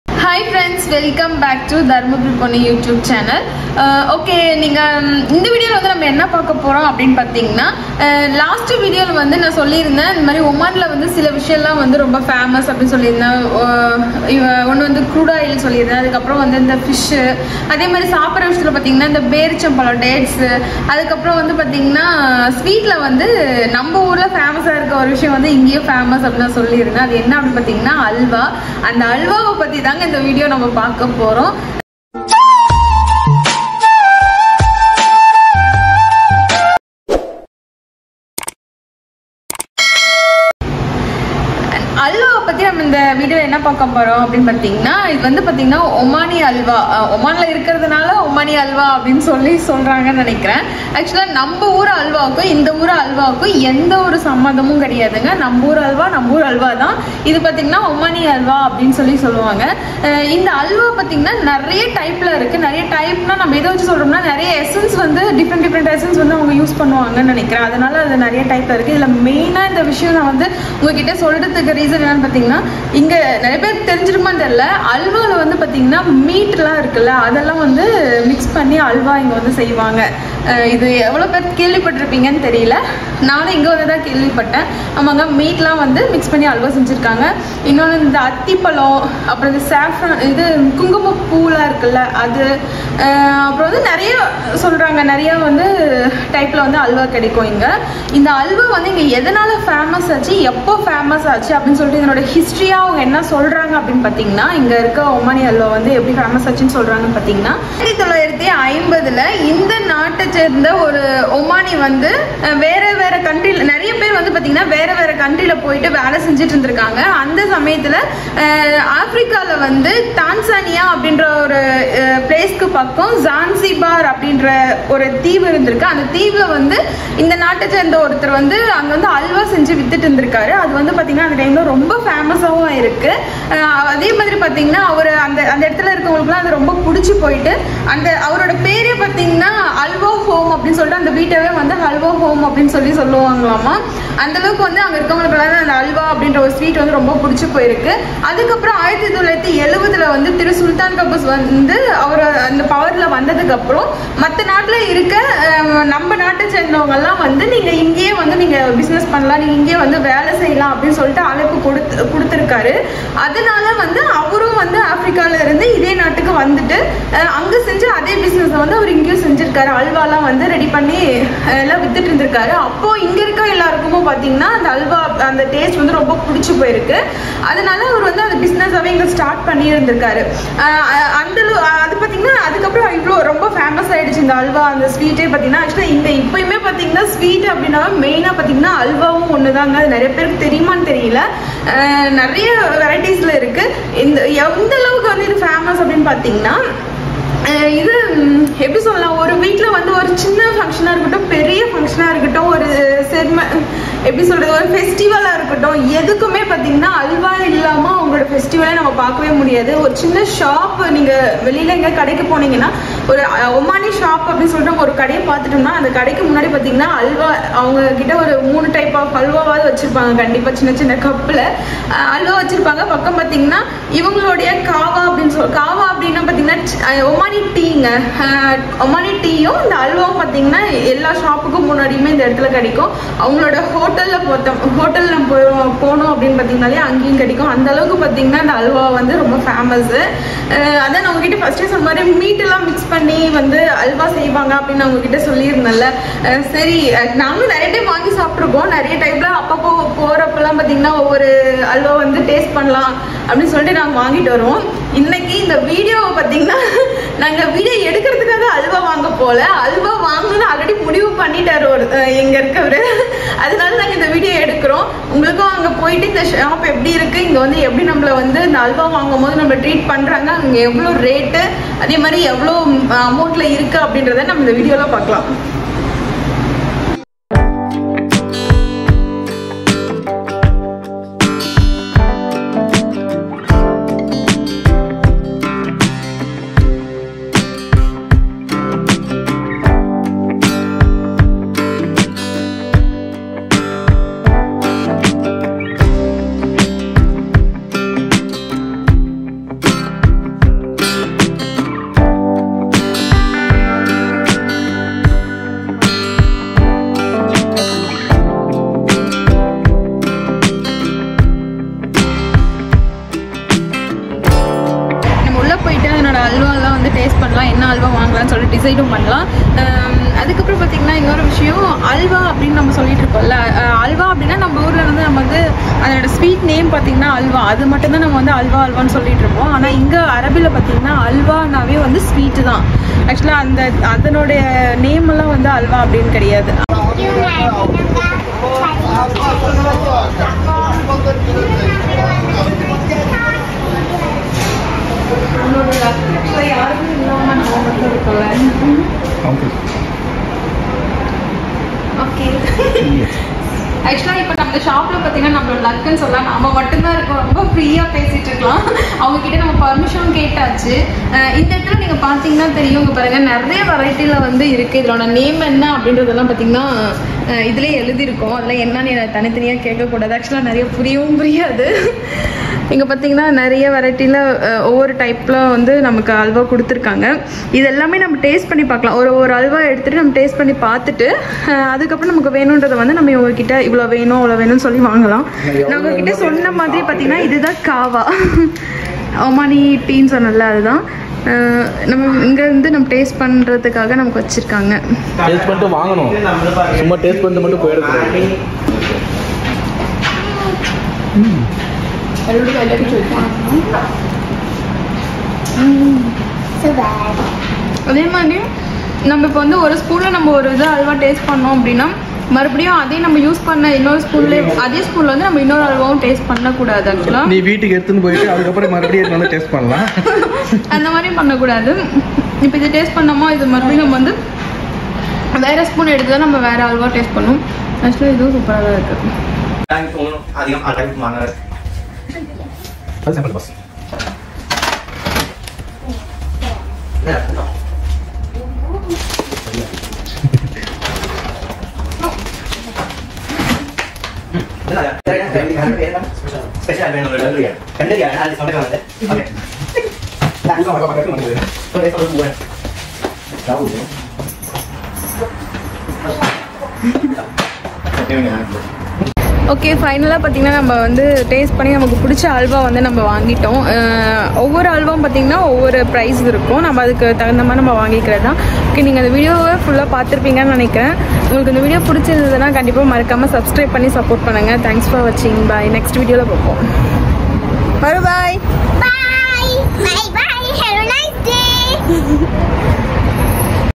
ஓமான் சுவையான ஹல்வா / Omani halwa / halwa with meat/ மாமிச ஹல்வா /Dharmapuri ponnu...
This video is about one of the best omani sweet...omani halwa..
its a traditional and famous sweet in Oman...
it will be served in all function and festival...
also liked by all people...
you must try the omani halwa when you visit oman...
@omani halwa...
@omani sweet...
@sweets of oman...
@muscat halwa...